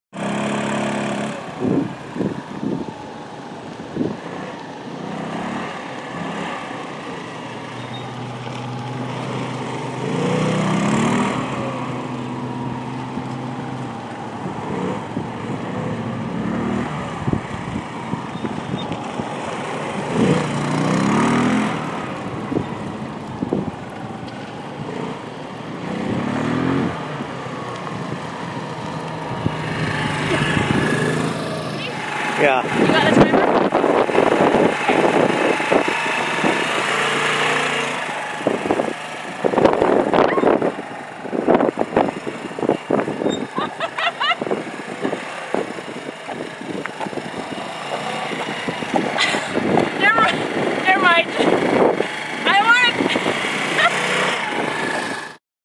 Parking Lot Slalom, 150cc Scooter Pod, 26.4 Seconds
This thing is crazy, but I just had to do the slalom with it.  I was worried it wouldn't be able to make the corners sharp enough, but there was no problem.  This baby can really turn!